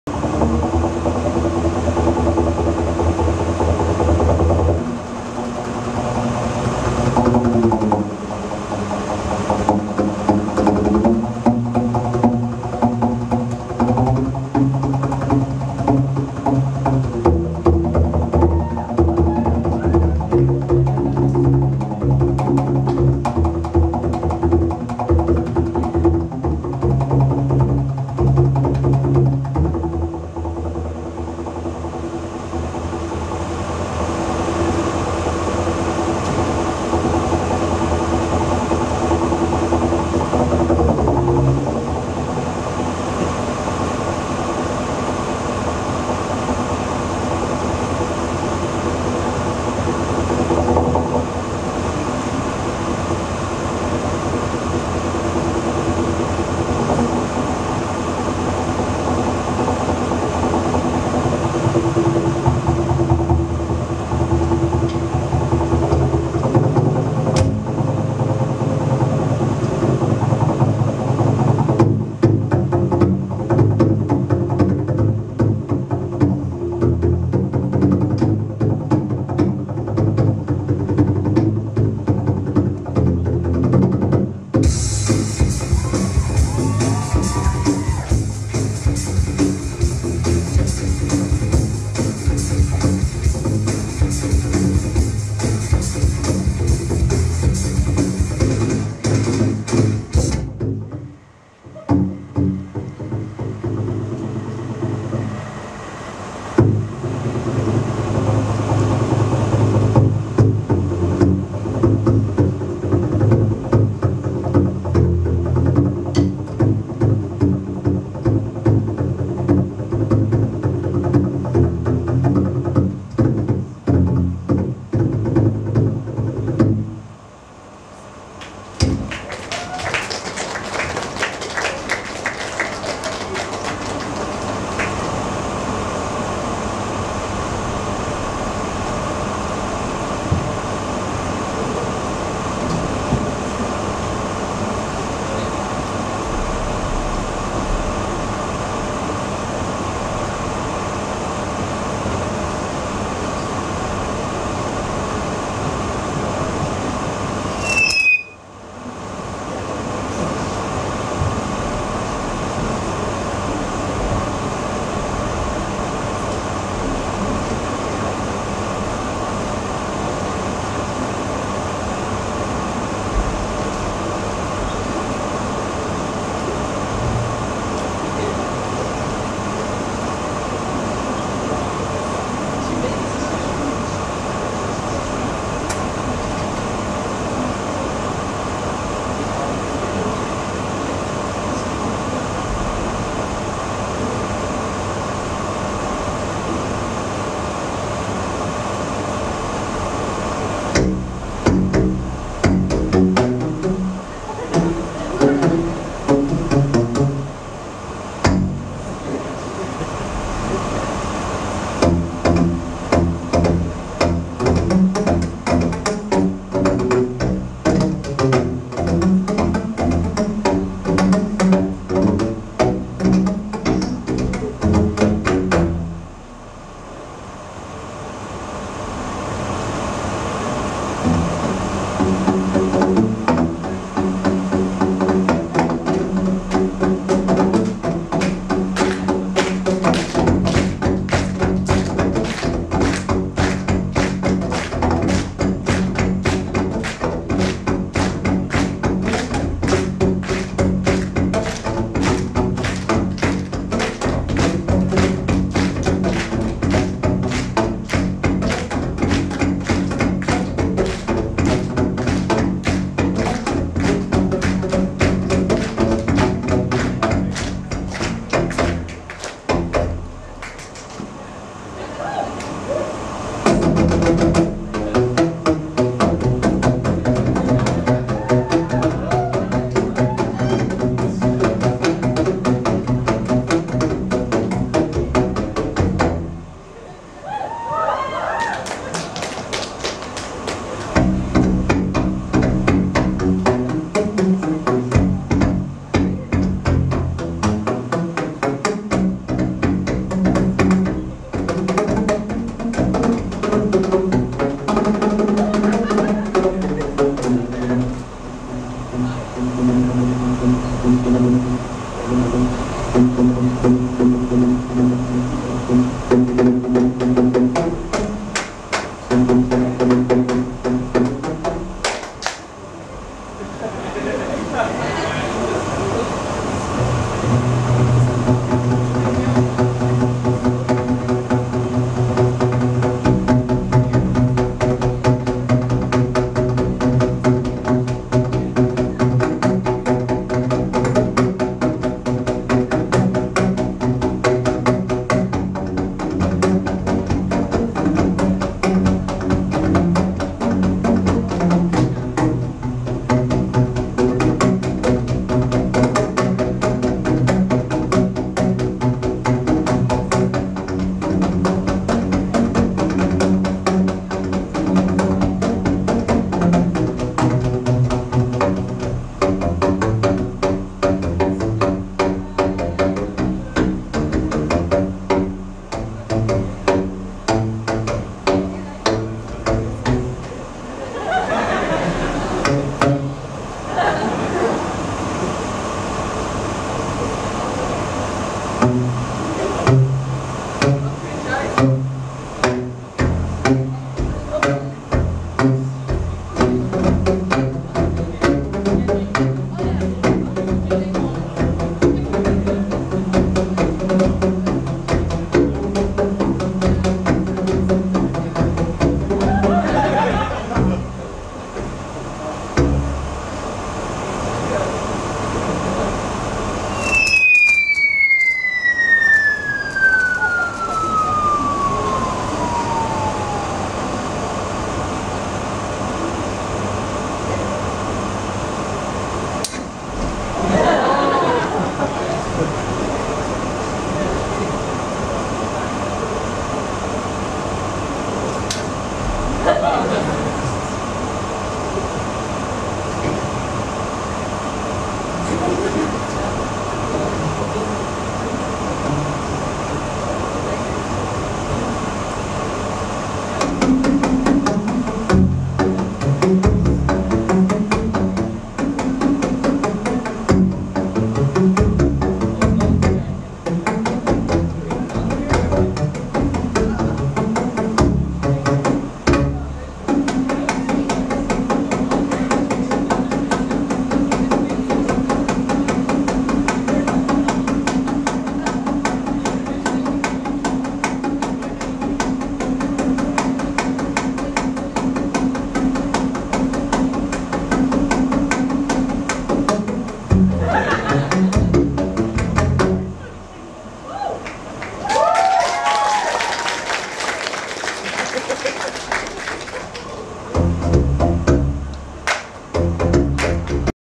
Blue Man Group Tribute - De Meester Almere
Blue Man Group Tribute in Poppodium de Meester Almere op zondag 25 mei 2014.